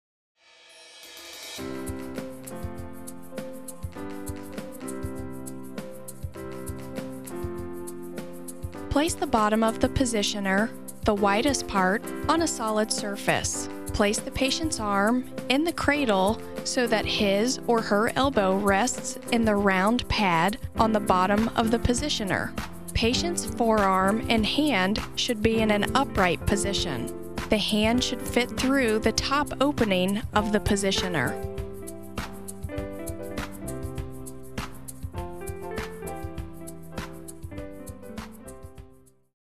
Arm Cradle
The DeRoyal Arm Cradle is used to help keep the arm in proper position to aid in circulation and to help prevent swelling due to injury or surgery.